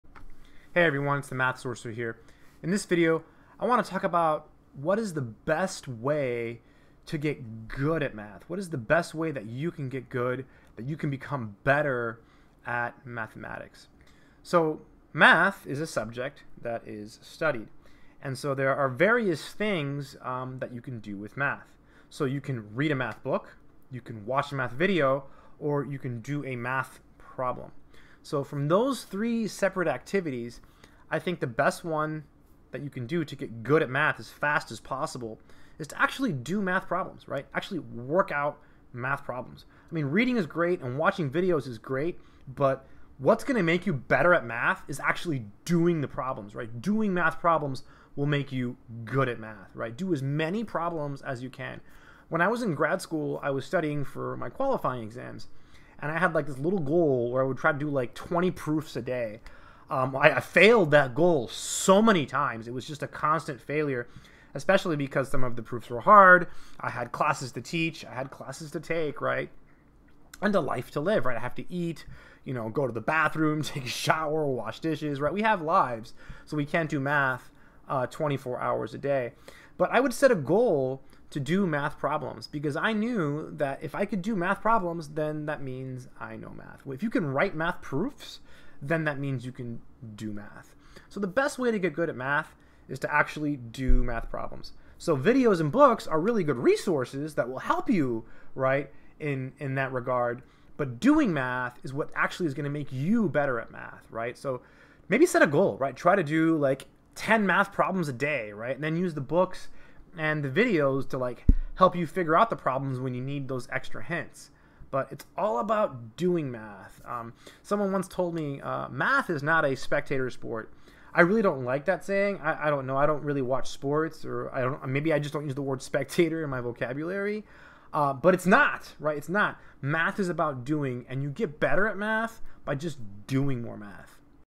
What is the Best Way to Get Good at Math?
What is the best way to good at math? In this video I talk about this briefly.

Please leave any comments or questions in the comment section below. Thank you:)